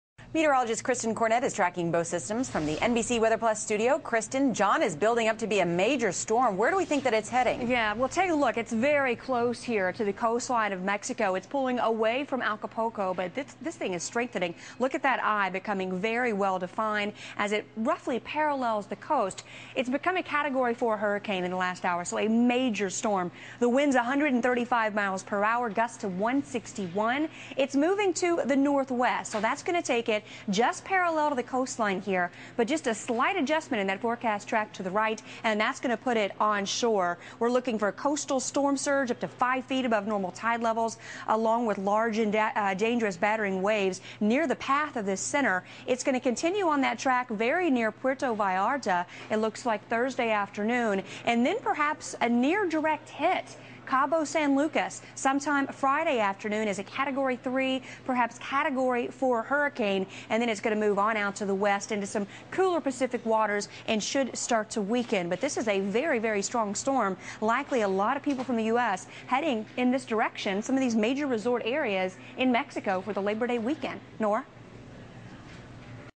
MSNBC (2006) - Tracking Hurricane John - 8/30/06
From "August 30th, 2006" on MSNBC - View a report from NBC Weather Plus Meteorologist Kristen Cornett about the category 4 storm heading to mexico.